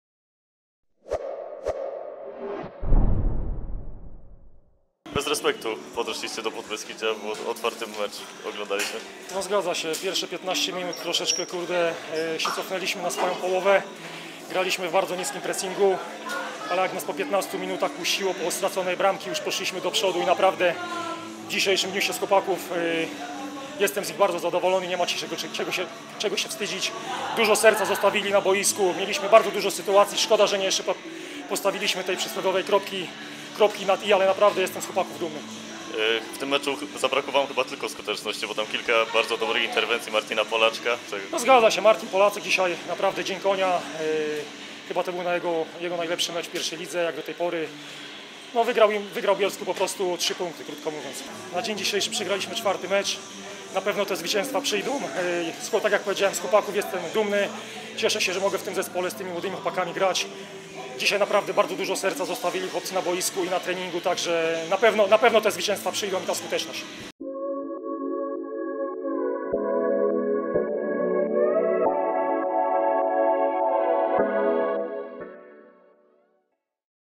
Podbeskidzie - Olimpia Grudziądz 2:1. Łukasz Sapela: "Jestem dumny z postawy drużyny"
W meczu inaugurującym 6. kolejkę 1. ligi Podbeskidzie Bielsko-Biała pokonało Olimpię Grudziądz 2:1. Dla gospodarzy było to przełamanie passy dwóch kolejnych porażek. Beniaminek, pomimo naprawdę dobrej, ofensywnej gry, zaliczył czwartą przegraną z rzędu. Po meczu do sytuacji Olimpii z optymizmem odniósł się bramkarz gości, Łukasz Sapela.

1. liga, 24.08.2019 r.
Podbeskidzie Bielsko-Biała - Olimpia Grudziądz 2:1 (1:0)
1:0 Komor 14'
2:0 Nowak 54'
2:1 Łukowski 80'